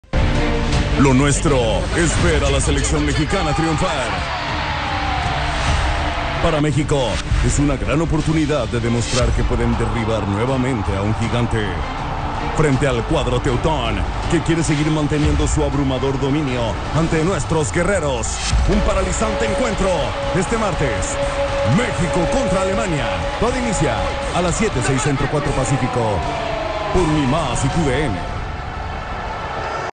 Mexico vs Alemania | Promo | TUDN 🇺🇸 y Unimas
Creditos a @tudn_usa y @Unimas

MexTour: Mexico vs Alemania
Martes 17 de Octubre
7P/6C/4PAC
Por TUDN y Unimas.